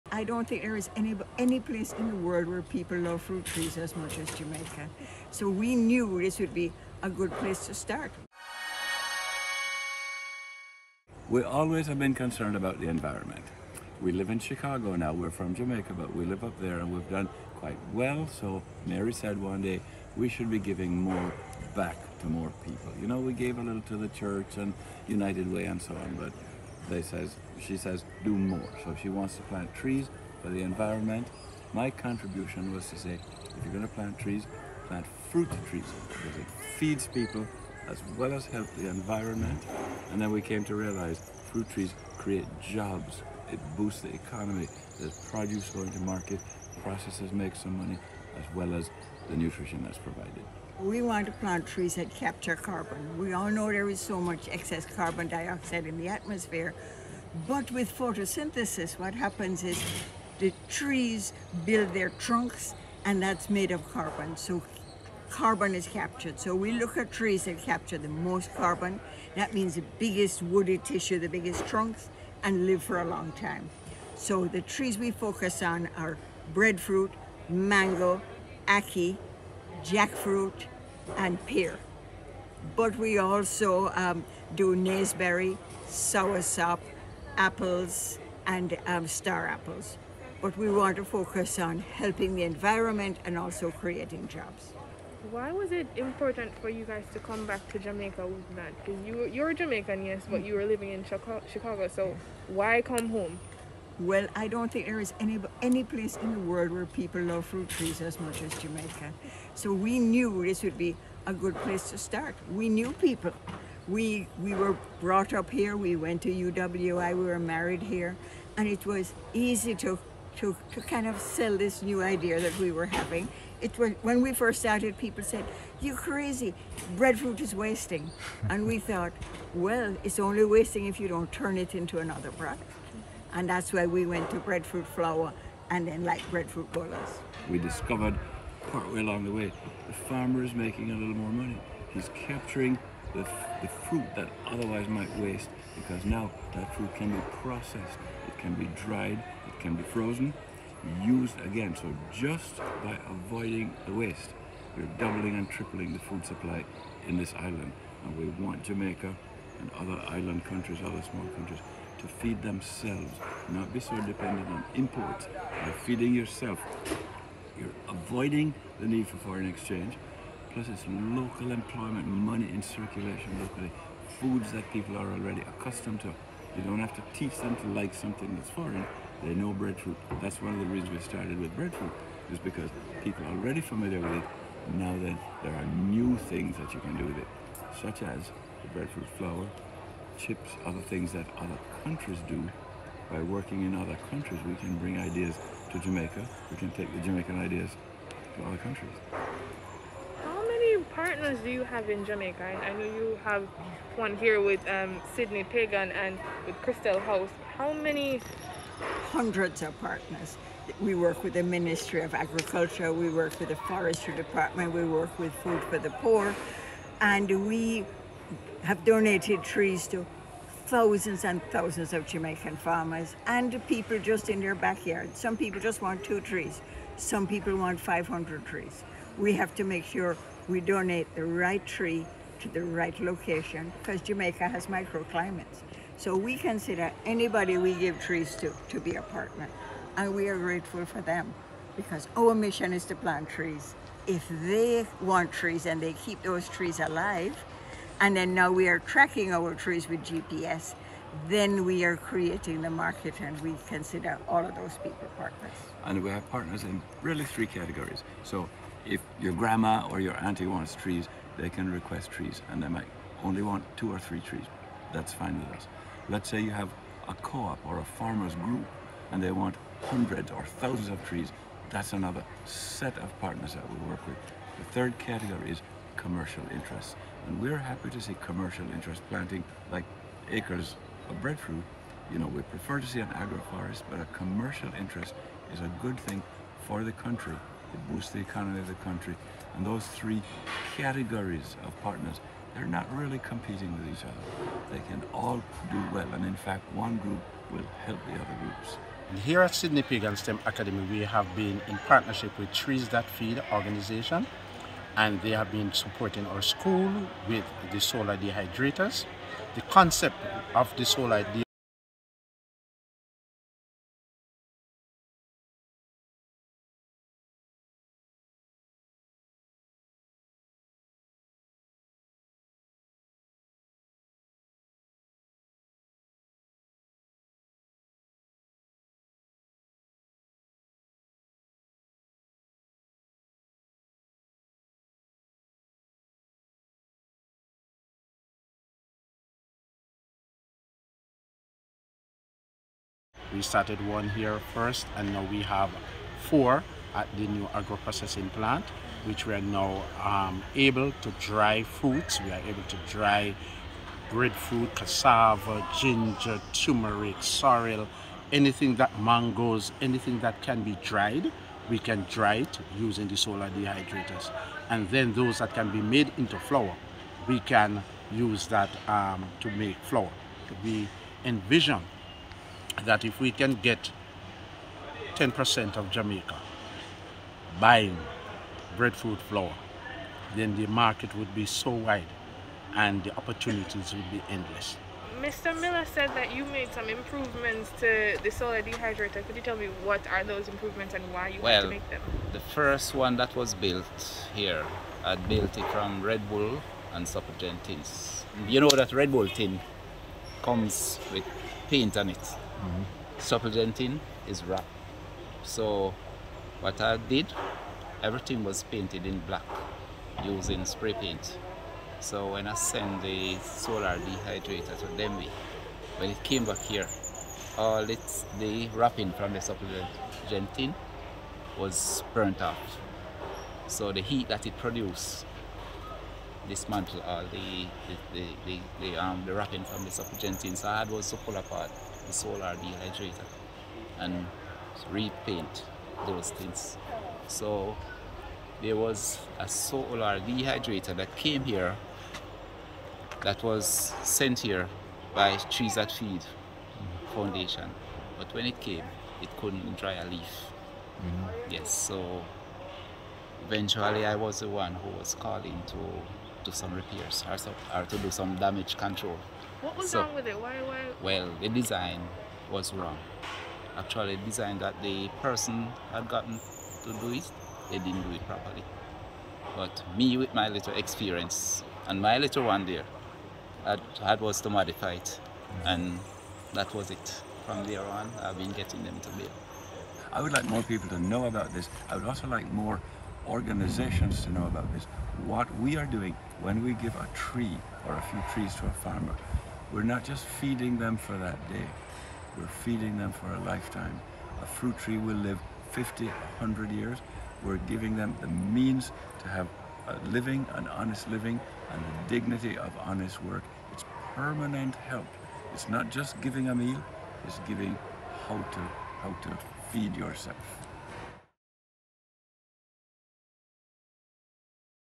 School uses sunlight to preserve food
The Sydney Pagon STEM Academy is leading the way to ending food waste through solar dehydration.
This is the process of preserving foods by drying them using the sun. While this process can occur naturally by leaving food like fruits and vegetables to the elements, using a solar dehydrator protects the food from bugs, dust and any debris that could contaminate it. Once the produce is dried, the food's shelf life is extended, allowing it to be further processed into a variety of products including flour. Principal of the St Elizabeth-based secondary institution, Milbert Miller, told THE WEEKEND STAR that the school has been experimenting with the science for several years, even entering its own prototype at the Denbigh Agricultural Show.
"The concept of the solar dehydrator at our school really began with our school entering at Denbigh competition using Red Bull cans. It worked so the concept wasn't new," he said. Though the prototype, which was built by maintenance supervisor Robin Rhoden, served them well, the school received a donation of seven solar dehydrators from philanthropic entity, Trees That Feed Foundation. Rhoden, who has dedicated his entire life to this form of food conservation and preservation, said this is the way Jamaicans ought to be going in terms of food preservation and conservation.